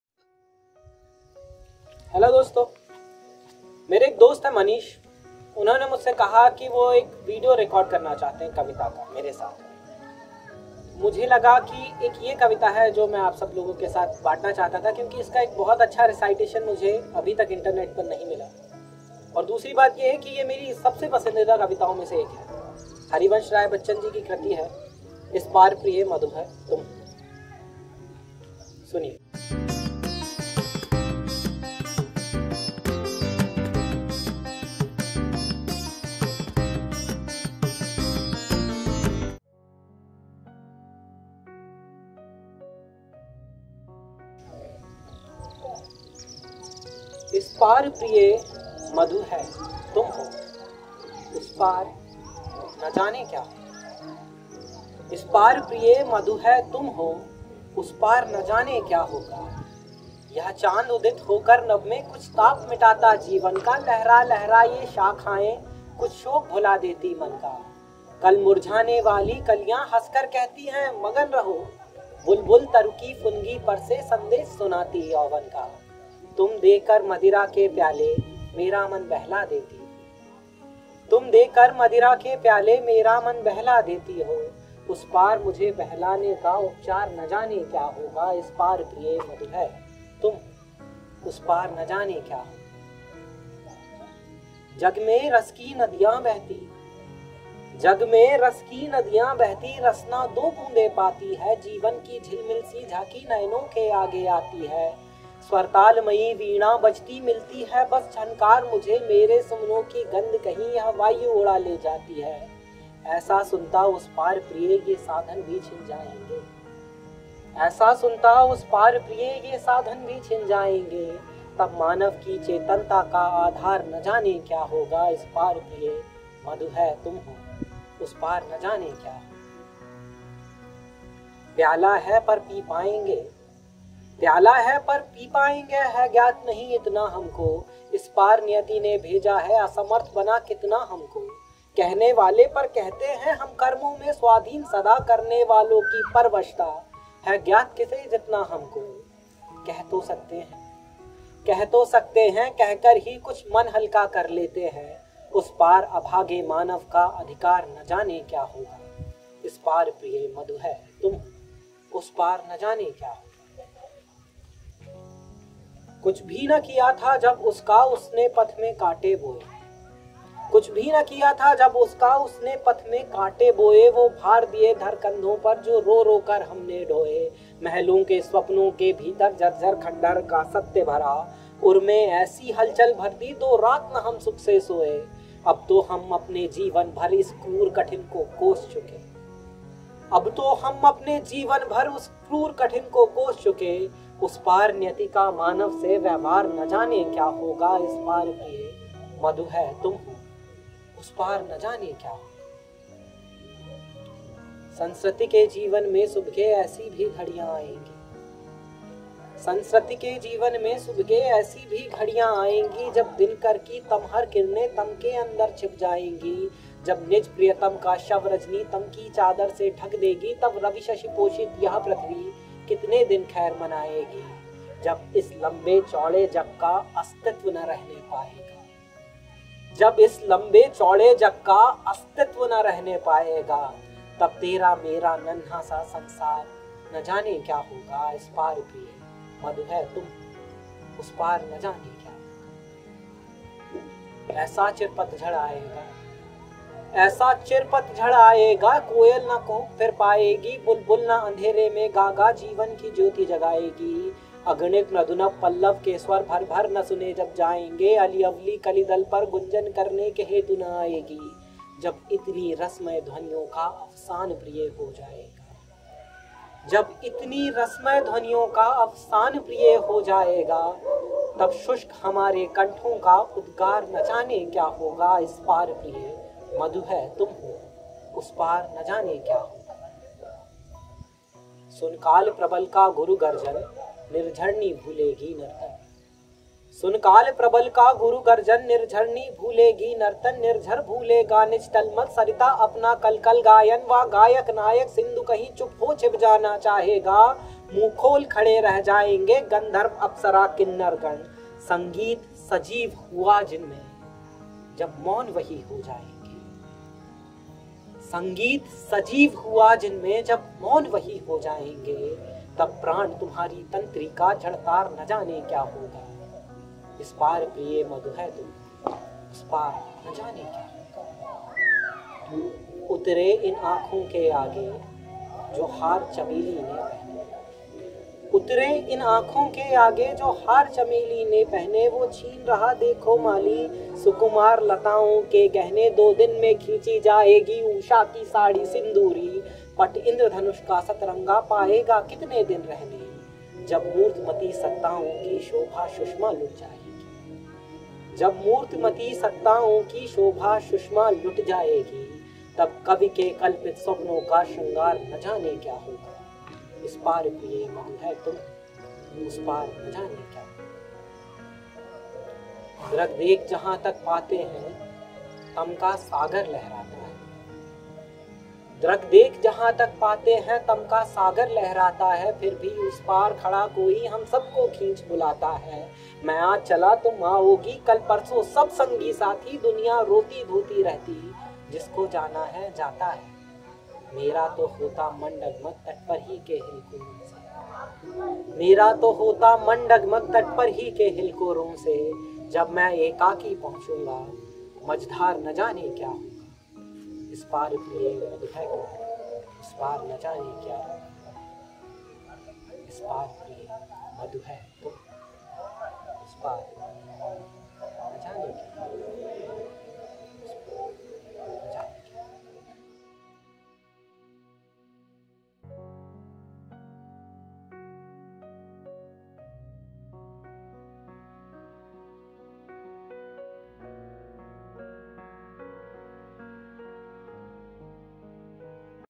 Kavitabol - Hindi - Harivansh Rai Bachchan - Ft. Naveen Chourey - Is Paar Priye
इस पार, प्रिये मधु है तुम हो - हरिवंशराय बच्चन

Watch Is Paar Priye Madhu Hai Tum Ho by Harivansh Rai Bachchan